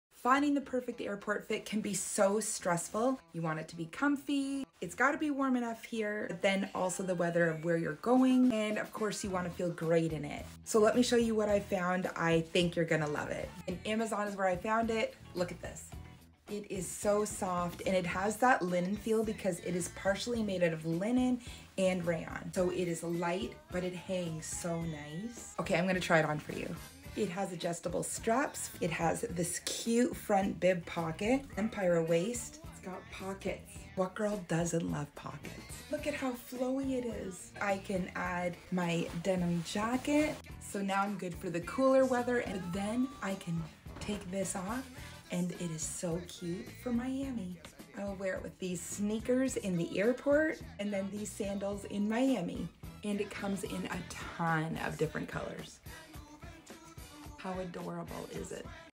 The CUTEST and most COMFY Airport Fit!
Stop your scroll and take a look at this cute airport fit!

 I might be the only one, but I always find it hard to find a good outfit that I look good in and can be comfortable for the long-haul!

This Amazon jumper is absolutely adorable!